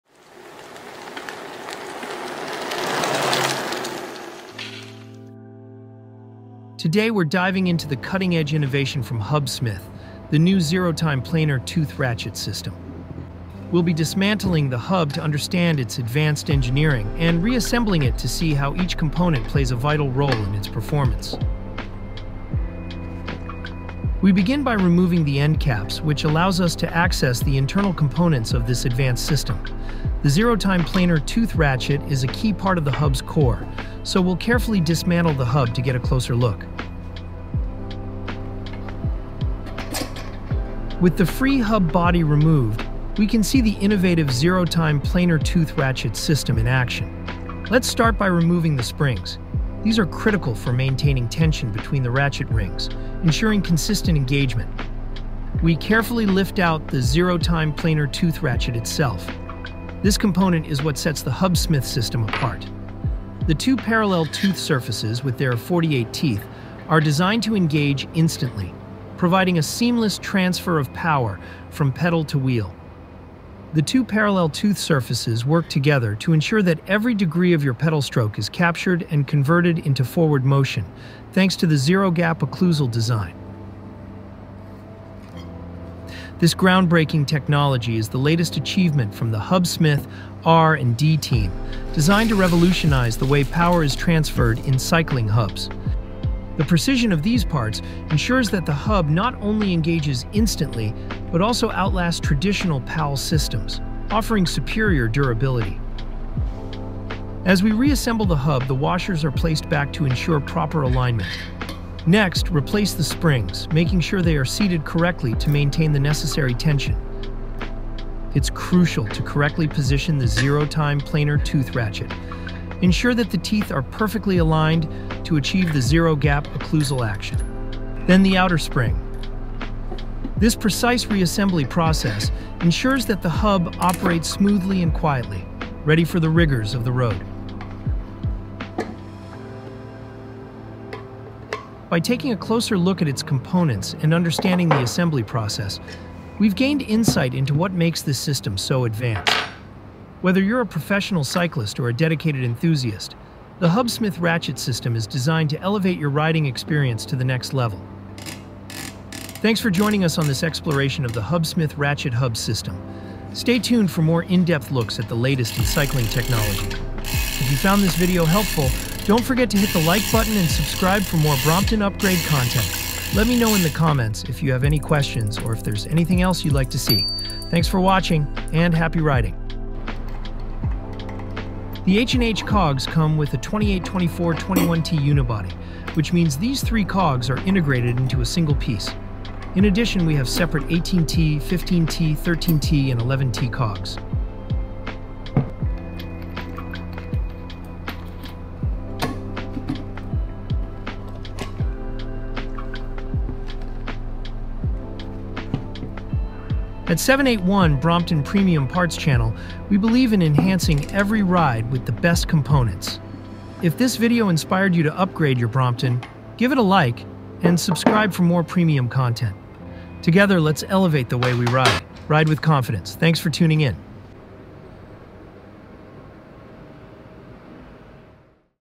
What's inside the Hubsmith Ratchet Hubs? Lets open it up!
Hubsmith has introduced a zero-time planar tooth ratchet for the ratchet body drive structure. This system features two parallel tooth surfaces with 48 teeth, achieving a zero-gap occlusal action during the full 360-degree driving process. This means that as you pedal, the ratchet engages instantly and fully, allowing for more direct power output without any delay or slack.

As we reassemble the hub, it’s crucial to correctly position the zero-time planar tooth ratchet first. Ensure that the teeth are perfectly aligned to achieve the zero-gap occlusal action. Next, replace the springs, making sure they are seated correctly to maintain the necessary tension. Then, the washers are placed back to ensure proper alignment, followed by the lock rings, which are tightened securely to hold everything in place.